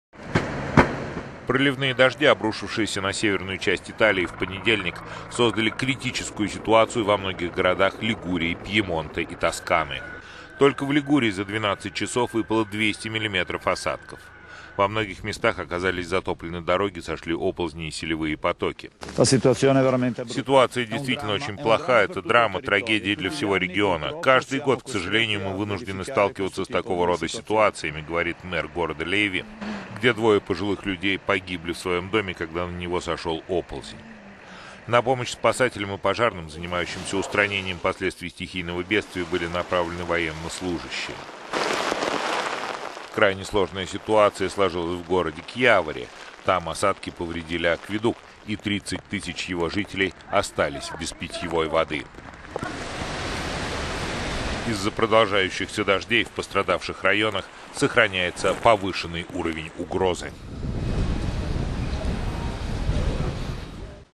Север Италии затопило
Проливные дожди обрушившиеся на северную часть Италии в понедельник, создали критическую ситуацию во многих городах Лигурии, Пьемонта, Тосканы.
Только в Лигурии за 12 часов выпало 200 мм осадков. Во многих местах оказались затоплены дороги, сошли оползни и селевые потоки.

"Ситуация действительно очень плоха, это драма, трагедия для всего региона. Каждый год, к сожалению, мы вынуждены сталкиваться с такого рода ситуациями", - говорит мэр городка Лейви, где двое пожилых людей погибли, в своем до…